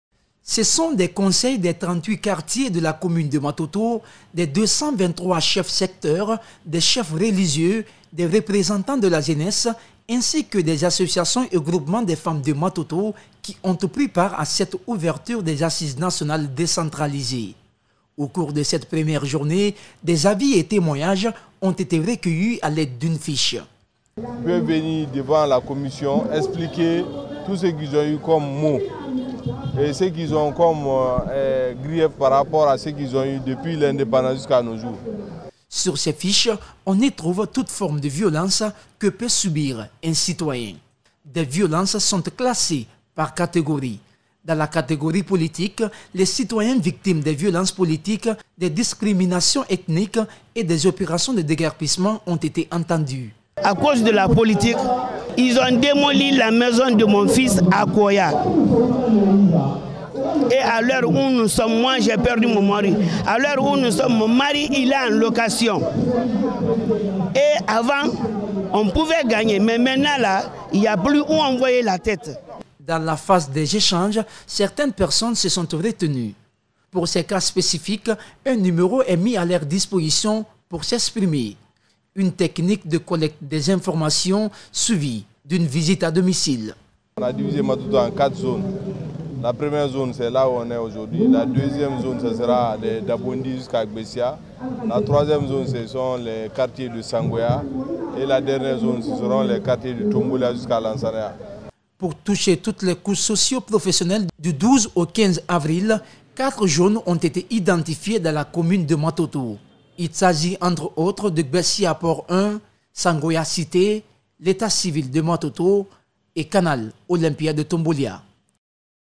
ASSISES NATIONALES - MATOTO : Le comité communale de matoto face aux citoyens
ASSISE NATIONALE - MATOTO : Le comité communale de matoto face aux citoyens